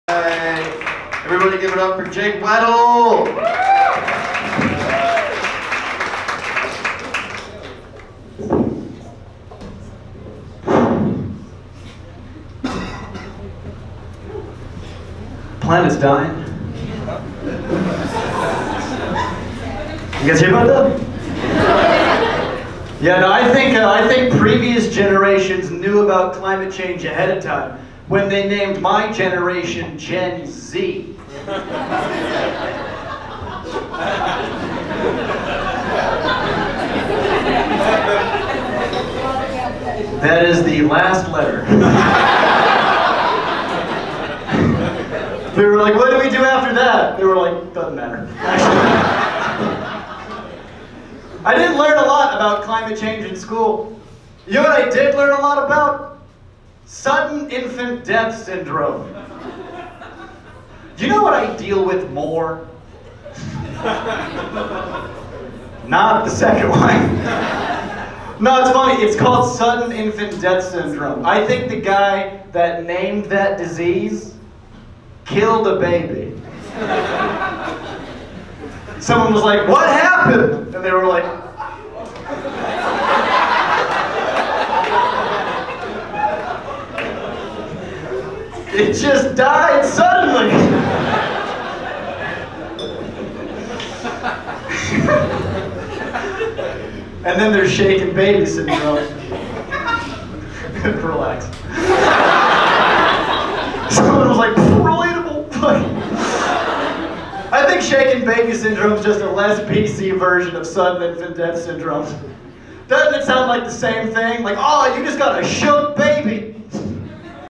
Jake Weddle: The Planet is Dying
Jake Weddle does stand up at Goodnights Comedy club